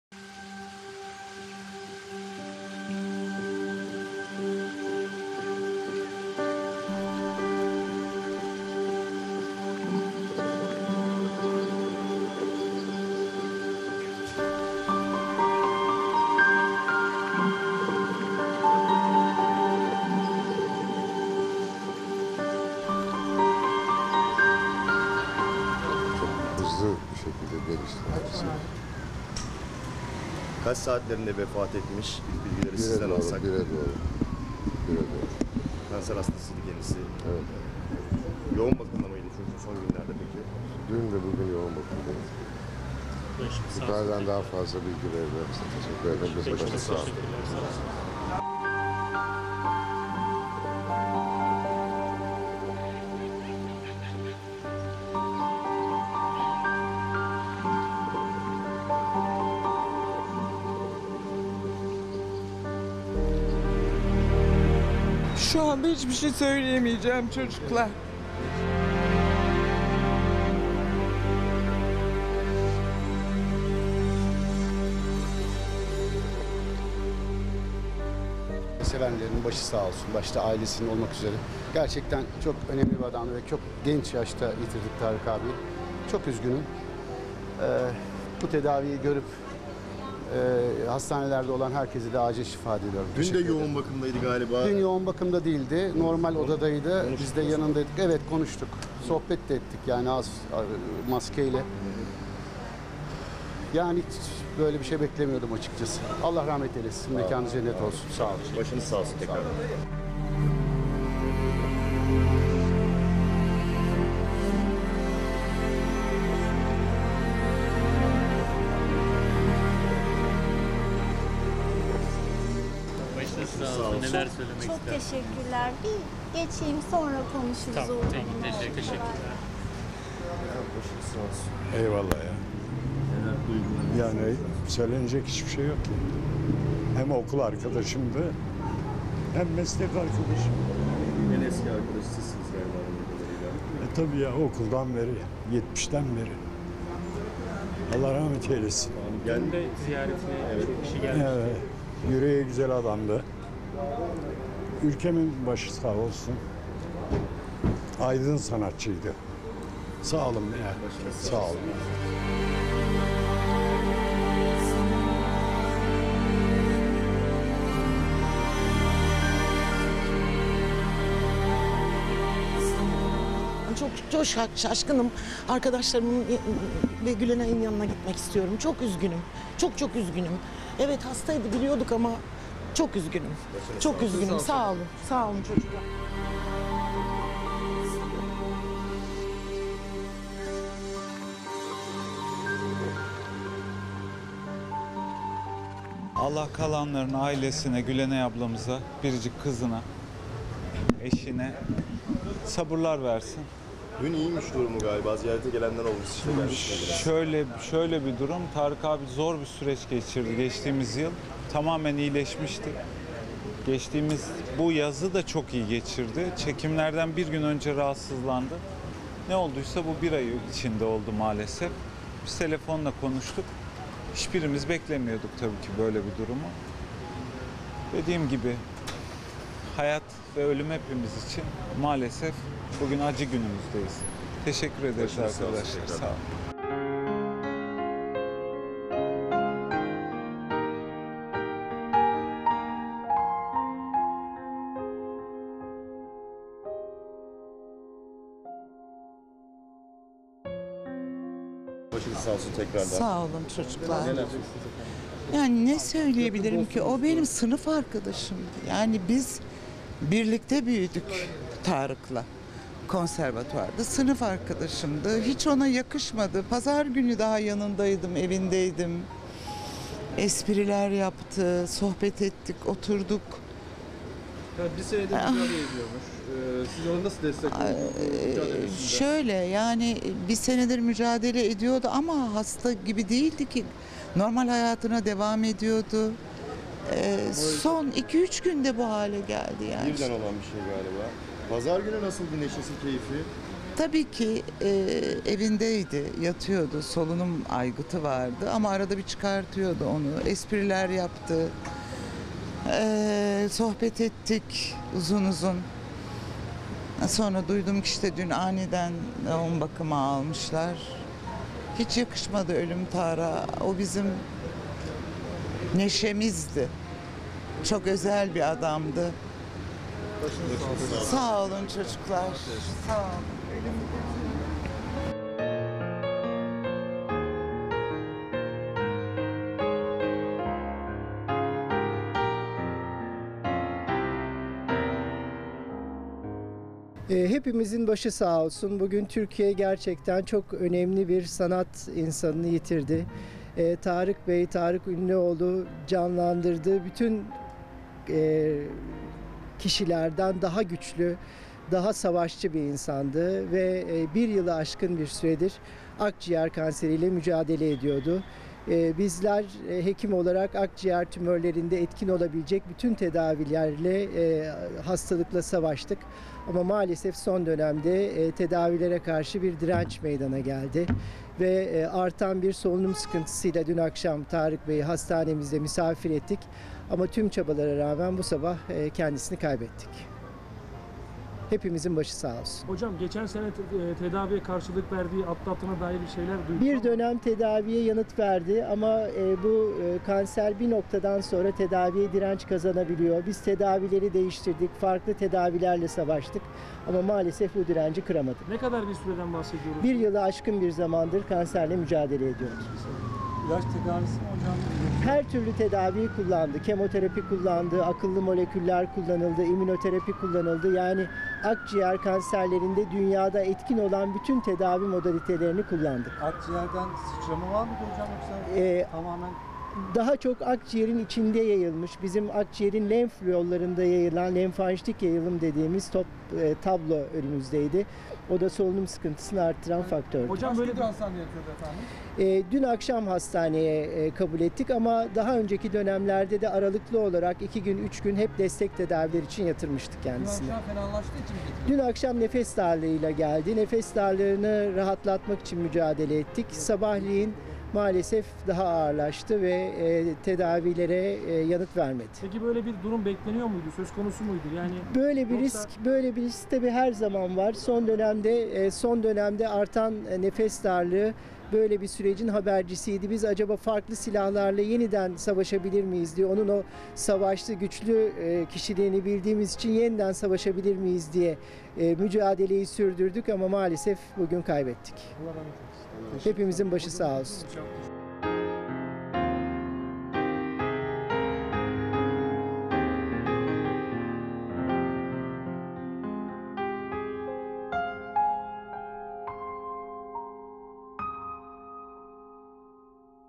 Tarık Ünlüoğlu'nun vefatının ardından hastaneye ünlü akını... Doktoru ölüm nedenini açıkladı!
Son olarak Eşkiya Dünyaya Hükümdar Olmaz dizisinde izlediğimiz usta oyuncu Tarık Ünlüoğlu bir süredir kanser tedavisi görüyordu. Bugün hayatını kaybetti... Hastaneye ünlüler akın etti.